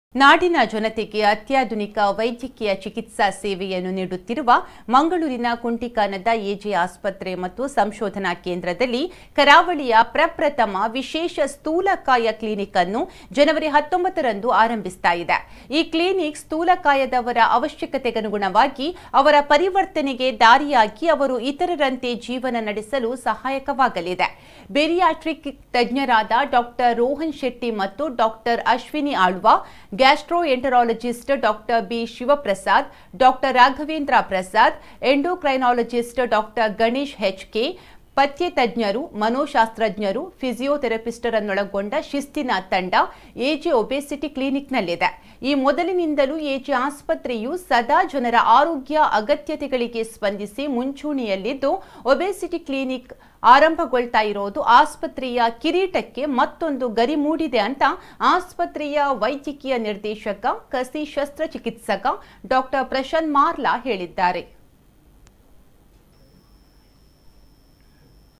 ನಾಡಿನ ಜನತೆಗೆ ಅತ್ಯಾಧುನಿಕ ವೈದ್ಯಕೀಯ ಚಿಕಿತ್ಸಾ ಸೇವೆಯನ್ನು ನೀಡುತ್ತಿರುವ ,ಮಂಗಳೂರಿನ ಕುಂಟಿಕಾನದ ಎ. ಜೆ. ಆಸ್ಪತ್ರೆ
ನಾಡಿನ ಜನತೆಗೆ ಅತ್ಯಾಧುನಿಕ ವೈದ್ಯಕೀಯ ಚಿಕಿತ್ಸಾ ಸೇವೆಯನ್ನು ನೀಡುತ್ತಿರುವ ಮಂಗಳೂರಿನ ಕುಂಟಿಕಾನದ ಎ. ಜೆ. ಆಸ್ಪತ್ರೆ ಮತ್ತು ಸಂಶೋಧನಾ ಕೇಂದ್ರದಲ್ಲಿ ಕರಾವಳಿಯ ಪ್ರಪ್ರಥಮ ವಿಶೇಷ ಸ್ಥೂಲಕಾಯ ಕ್ಲಿನಿಕ್‍ನ್ನು ಜ. 19ರಂದು ಆರಂಭಿಸುತ್ತಿದೆ. ಈ ಕ್ಲಿನಿಕ್ ಸ್ಥೂಲಕಾಯದವರ ಅವಶ್ಯಕತೆಗನುಗುಣವಾಗಿ ಅವರ ಪರಿವರ್ತನೆಗೆ ದಾರಿಯಾಗಿ ಅವರು ಇತರರಂತೆ ಜೀವನ ನಡೆಸಲು ಸಹಾಯಕವಾಗಲಿದೆ. .ಬೇರಿಯಾಟ್ರಿಕ್ ತಜ್ಞರಾದ ಡಾ. ರೋಹನ್ ಶೆಟ್ಟಿ ಮತ್ತು ಡಾ. ಅಶ್ವಿನ್ ಆಳ್ವ, ಗ್ಯಾಸ್ಟ್ರೋ-ಎಂಟ-ರಾಲಜಿಸ್ಟ್ ಡಾ. ಬಿ. ಶಿವಪ್ರಸಾದ್, ಡಾ. ರಾಘವೇಂದ್ರ ಪ್ರಸಾದ್, ಎಂಡೋ-ಕ್ರೈ-ನಾಲಜಿಸ್ಟ್ ಡಾ. ಗಣೇಶ್ ಹೆಚ್. ಕೆ, ಪಥ್ಯೆ ತಜ್ಞರು, ಮನೋಶಾಸ್ತ್ರಜ್ಞರು, ಫಿಸಿಯೋ-ಥೆರಪಿಸ್ಟ್‍ರನ್ನೊಳಗೊಂಡ ಶಿಸ್ತಿನ ತಂಡ ಎ. ಜೆ. ಒಬೆಸಿಟಿ ಕ್ಲಿನಿಕ್‍ನಲ್ಲಿದೆ. ಈ ಮೊದಲಿನಿಂದಲೂ ಎ. ಜೆ. ಆಸ್ಪತ್ರೆಯು ಸದಾ ಜನರ ಆರೋಗ್ಯ ಅಗತ್ಯತೆಗಳಿಗೆ ಸ್ಪಂದಿಸಿ ಮುಂಚೂಣಿಯಲಿದ್ದು